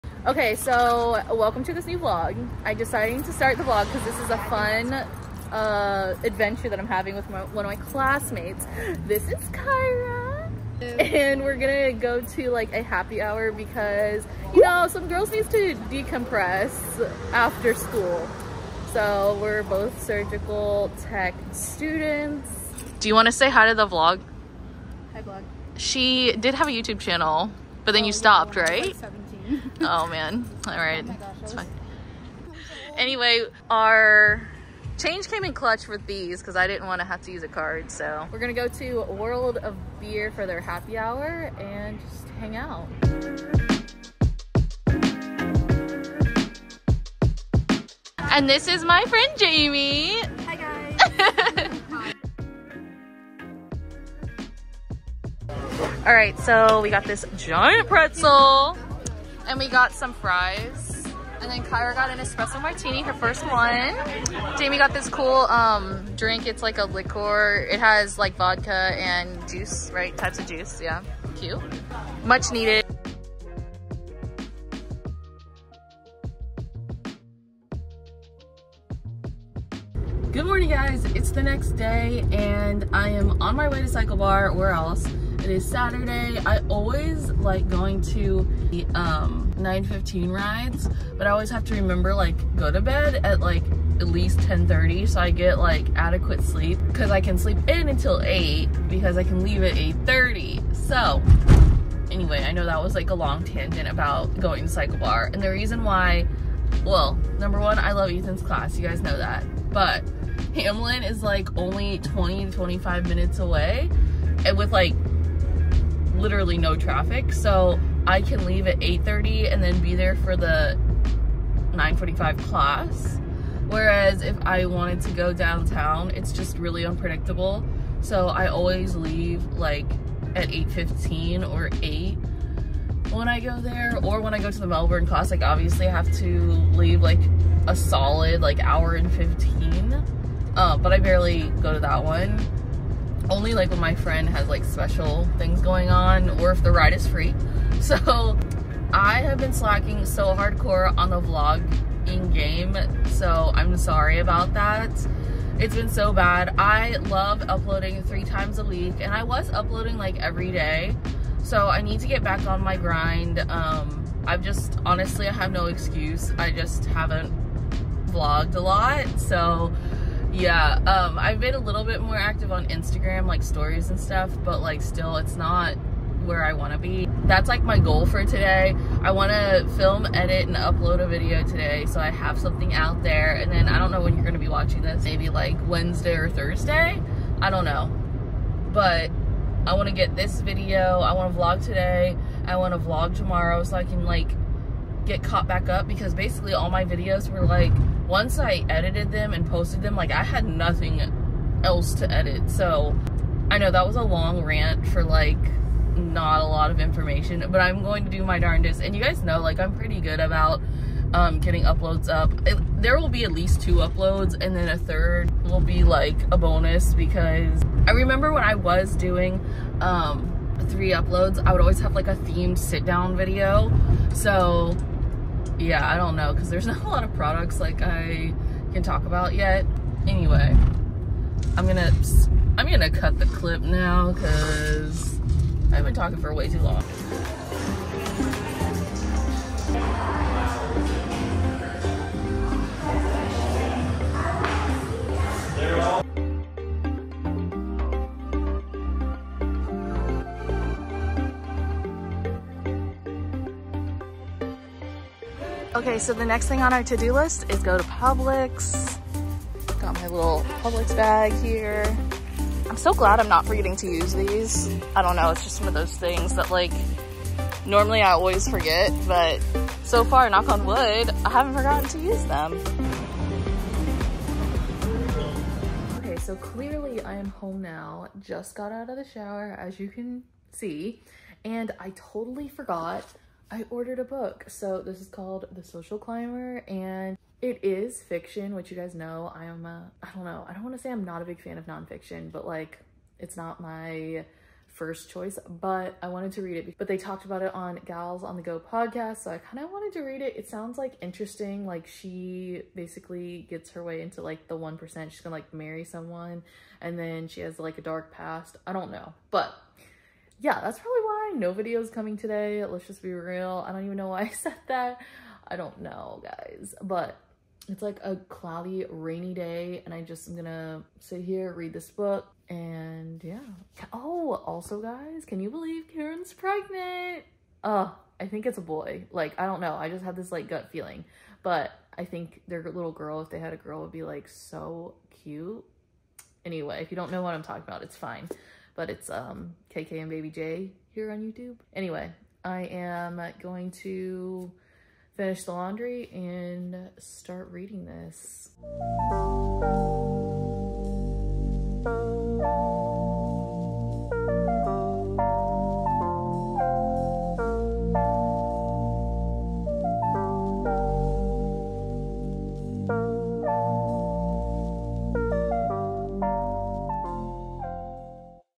life lately: meet my school friends + i got a new book!
✩ life lately: meet my school friends + i got a new book! I haven't picked up the camera in a bit, but here are some fun clips I got with some of my school friends. I will get back into vlogging now just been lazy lol.

❥  Alex